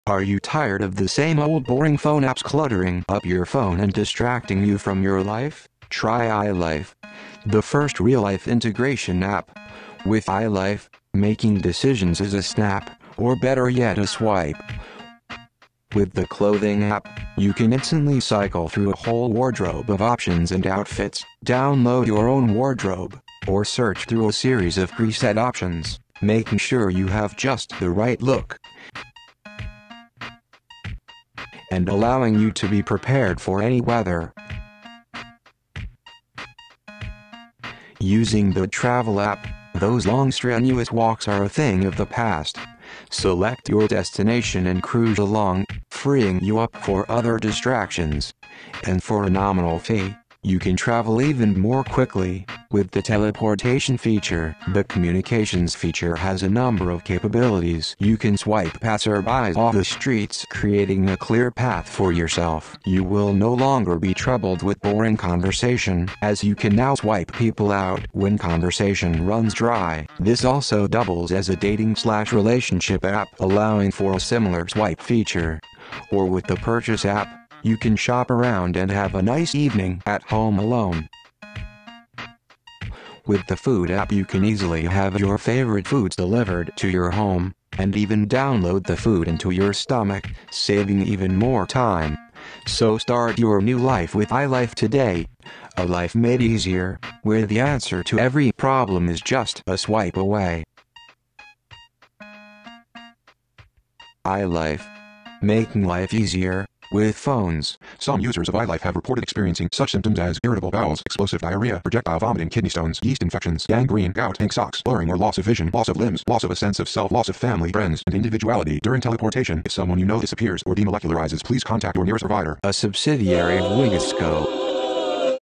iLife App Commercial
This is a fake commercial that I made, using pixelation animation. I made this for my Intro to Digital Photography class at RISD. This was my final project. I don’t feel that I’m that great at photography itself, so I decided to make an animation, using photography. The commercial is for a fake product, iLife, that is supposed to offer features, to help with your everyday life. The features, are supposed to free you up, to have more time to waste on your cell phone.